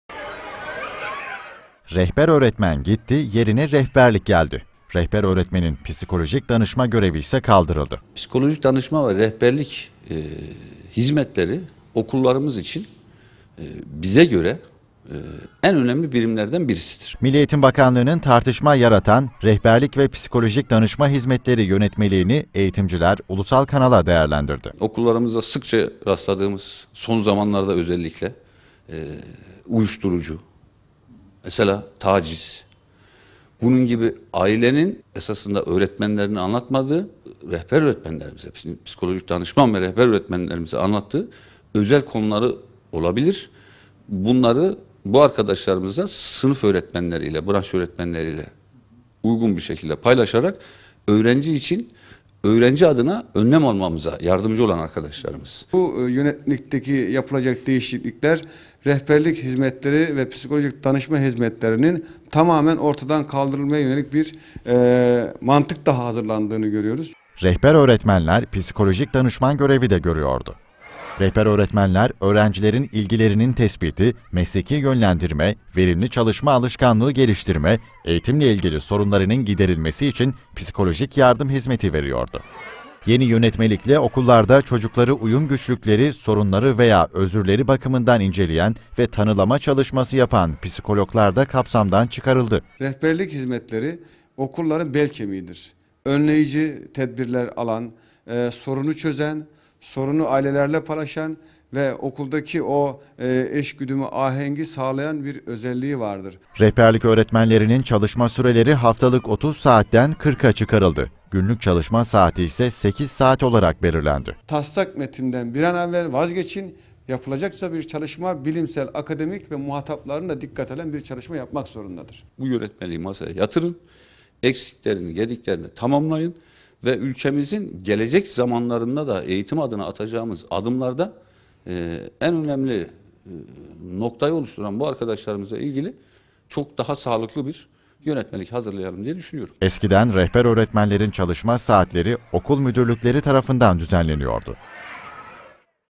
Eğitimciler MEB'e seslendi: Tekrar gözden geçirin
Eğitimciler, Milli Eğitim Bakanlığı’nın “rehberlik ve psikolojik danışma hizmetleri yönetmeliği”nin tekrar gözden geçirilmesini istedi. Çünkü, "Psikolojik danışma servisleri” kaldırıldı. Bu yönetmelikle öğrencilere ve ailelerine yönelik psikolojik danışma hizmeti verilmeyecek.